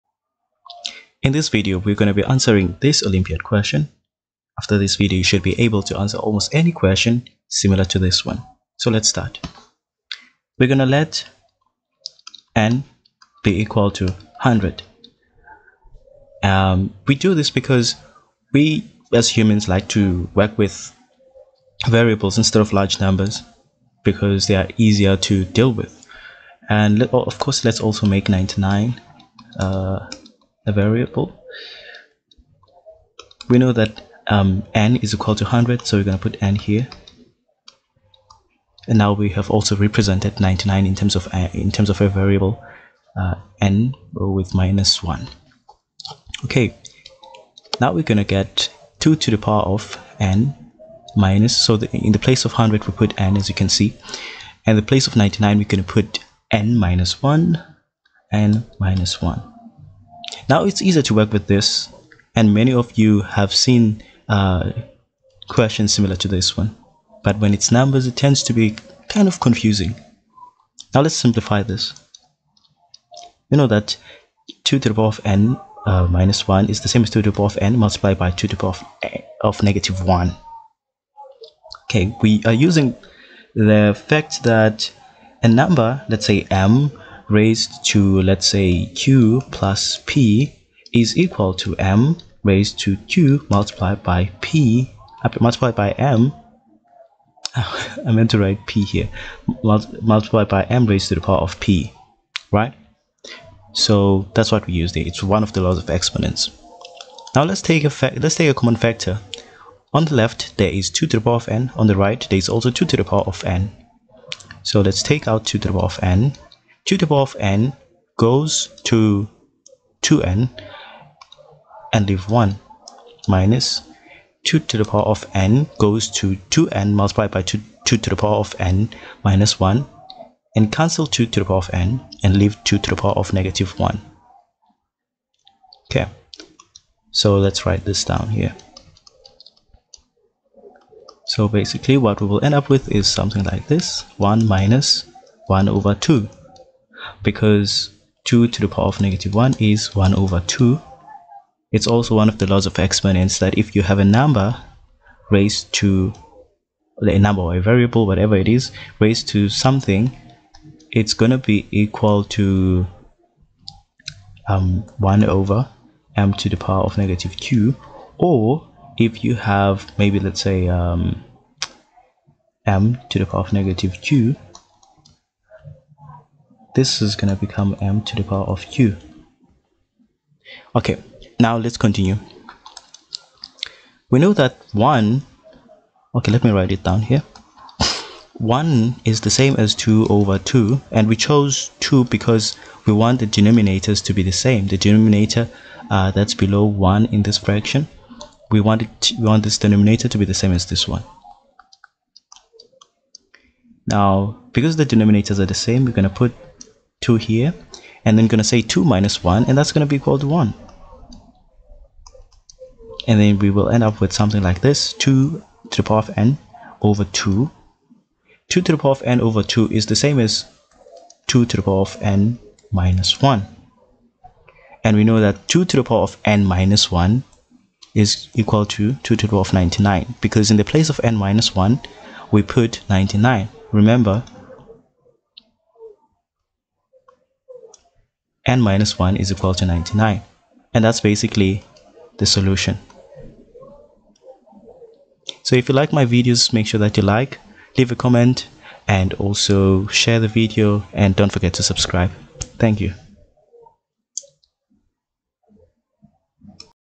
How to find the value of 2^100 - 2^99 | Maths Olympiad
In this video I'm providing a general method often used to find the value of numbers with large powers(exponents). I'm finding the value of 2^100-2^99